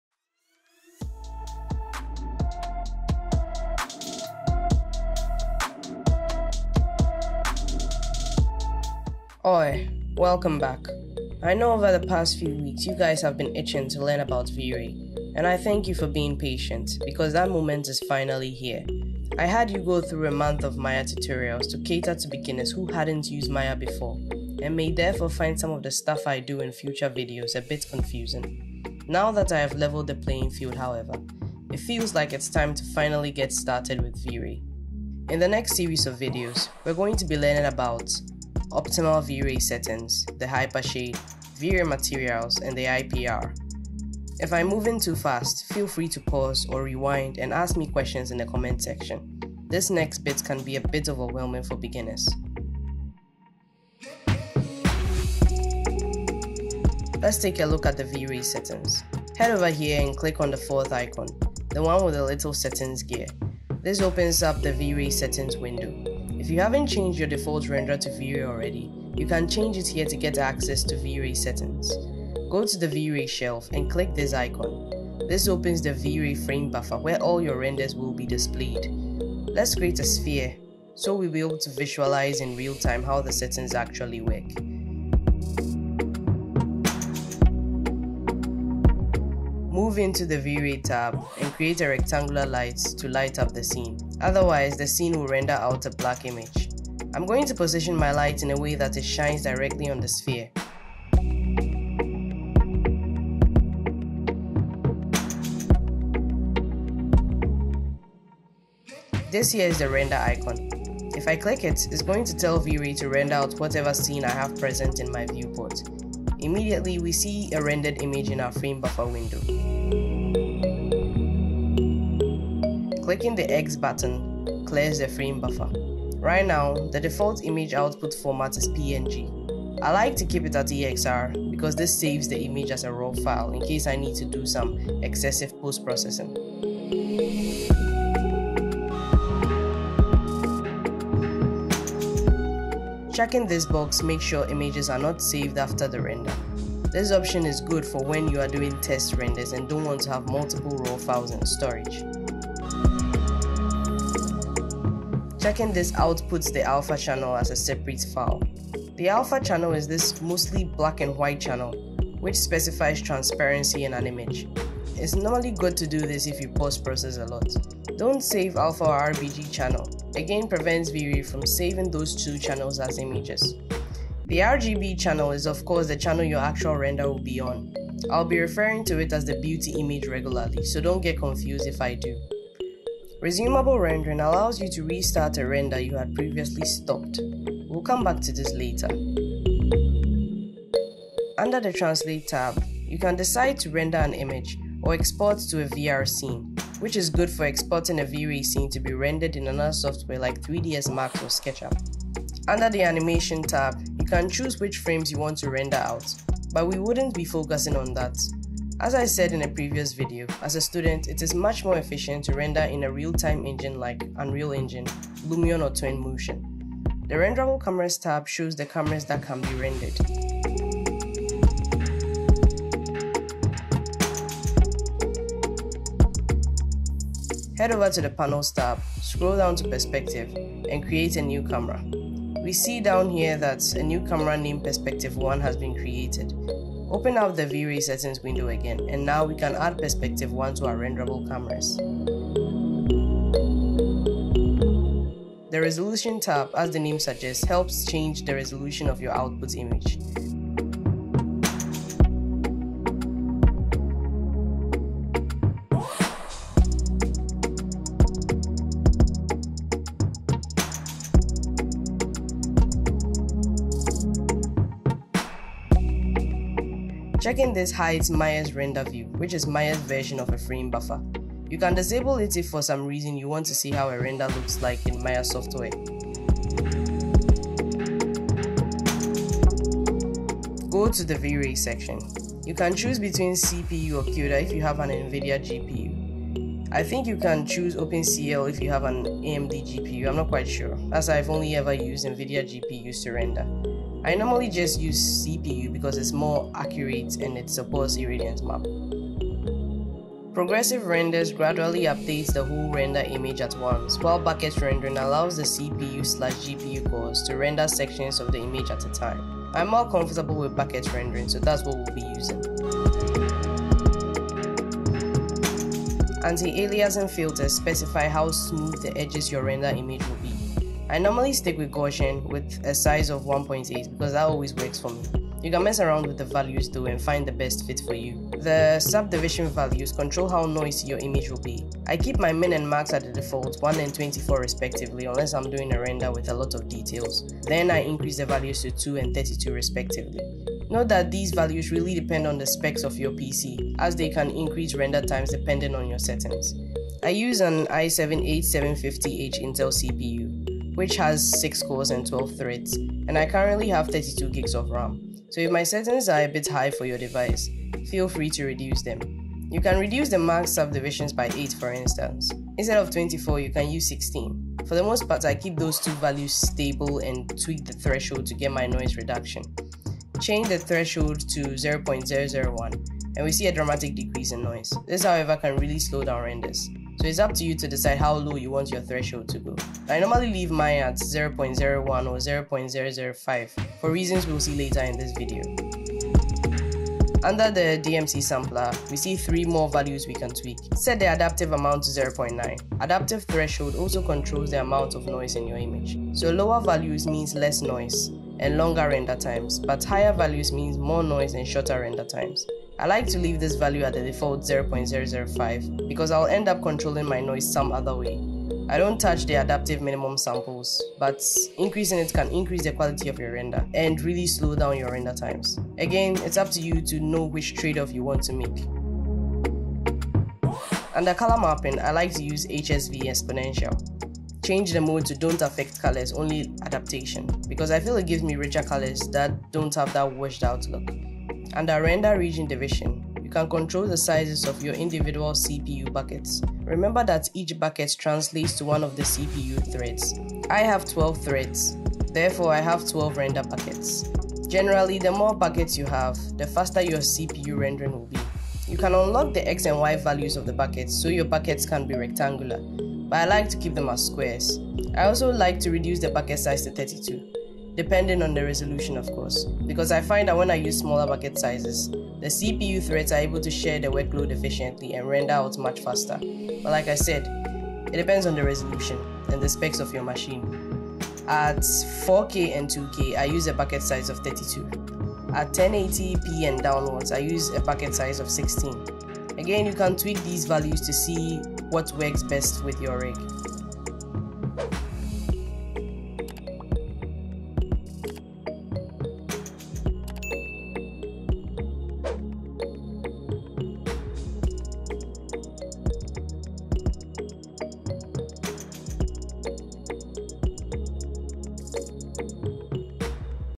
VRay Render Settings | Beginner's Guide To Vray For Maya
Today we're going to take a look at the render settings window. You'll understand all those cryptic VRay settings and what each one does. You'll also know the best values to use for faster, prettier renders.